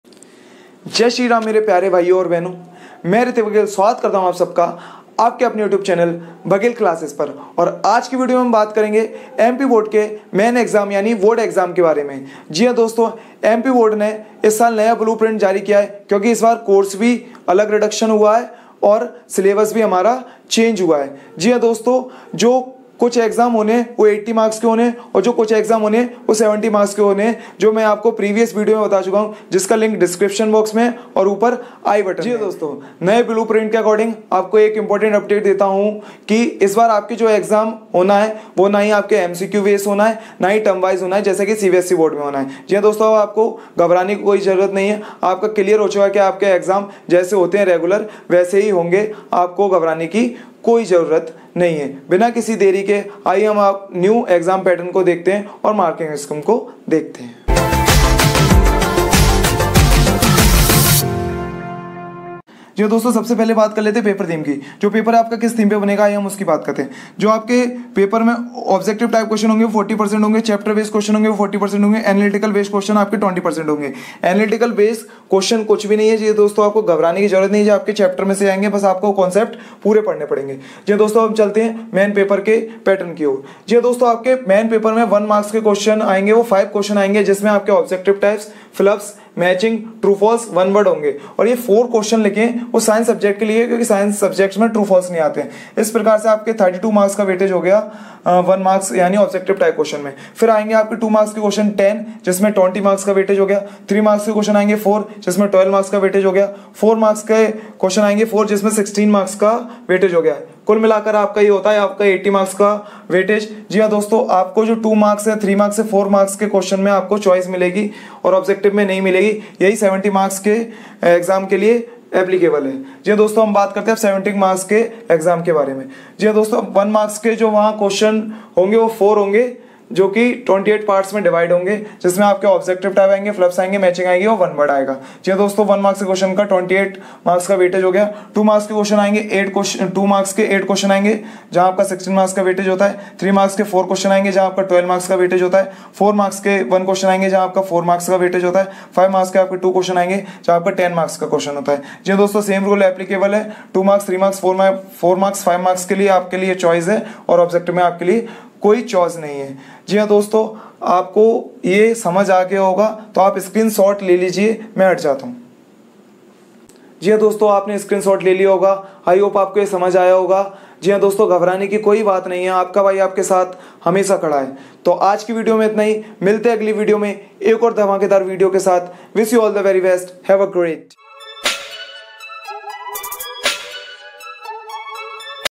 MP Board Exam Pattern Class 9th to 12th 2021-22 I MP Board New Blue Print 2022
Hey, My Dear Friend!

In this video, you will get the info about the Exam Pattern of Class 9-12  MP Board .Plz, watch this video till the end to get full details...
(इस वीडियो में MP Board द्वारा कक्षा 9-12 का परीक्षा पैटर्न है)